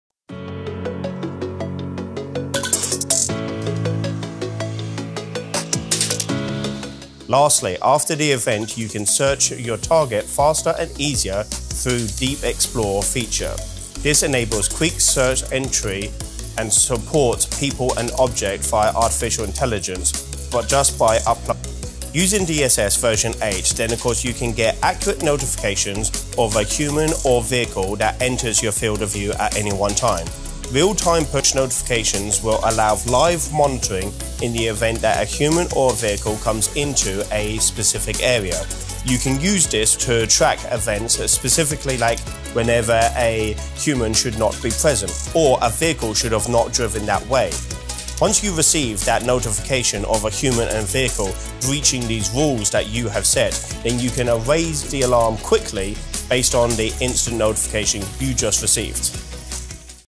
06 DSS V8 Deep Xplore Objects and People
Lastly after the event you can search for your target faster and easier through our “DeepXplore” feature. This enables Quick search entry, and Integrated Searches, . You can search “People” and “Objects” via Artificial Intelligence by just uploading an image into the system. This implements intelligent retrieval of targets

Find lost or who took stolen objects from your company / organization via Artificial Intelligence.